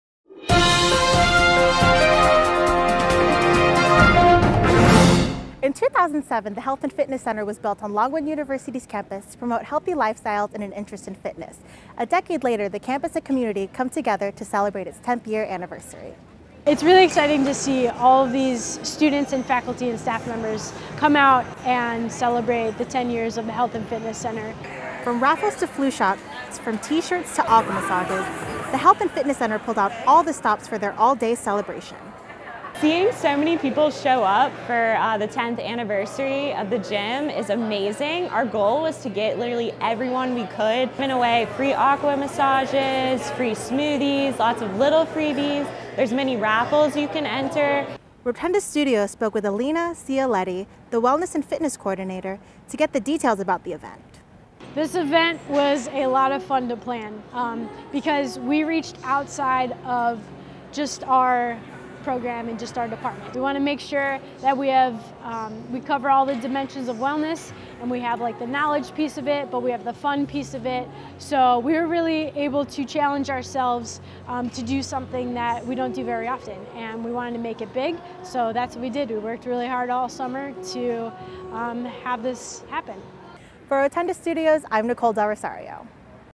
The Health and Fitness Center celebrate their 10th anniversary
Rotunda Studios' Nicole Del Rosario covers the tenth anniversary of the Health and Fitness Center.

Executive Producer: Payton Conway
Videographer: Payton Conway